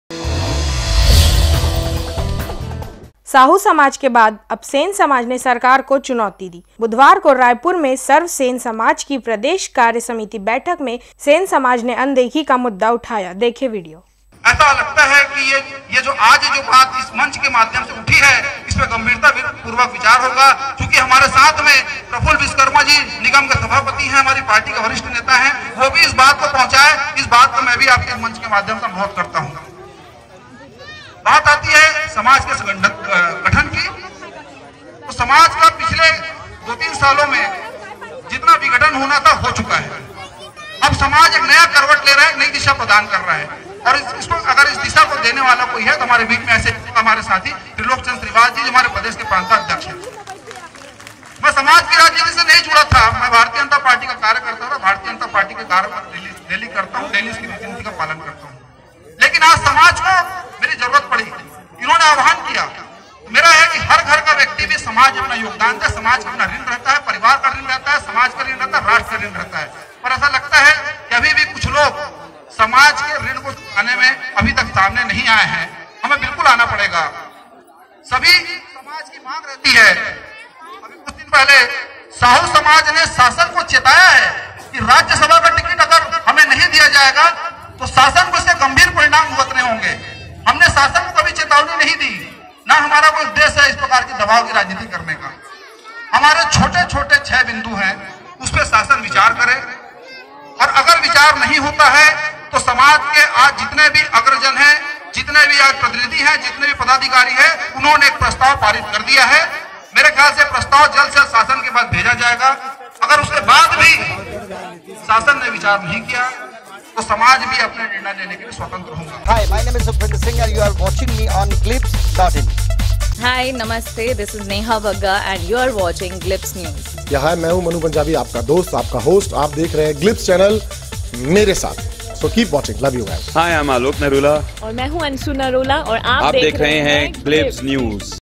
साहू समाज के बाद अब सेन समाज ने दी सरकार को चुनौती, अनदेखी का मुद्दा उठा
रायपुर। साहू समाज के बाद अब सेन समाज ने सरकार को चुनौती दी। बुधवार को रायपुर में सर्व सेन समाज की प्रदेश कार्य समिति की बैठक मे सेन समाज की अनदेखी का मुद्दा उठा।  सभा में समाज के सभी पदाधिकारी एवं 25 जिले के अध्यक्ष शरीक हुए और समाज की अनदेखी का आरोप भाजपा पर लगाया गया। मंच मे खुद भाजपा नेता गौरीशंकर श्रीवास ने सरकार को चेता डाला।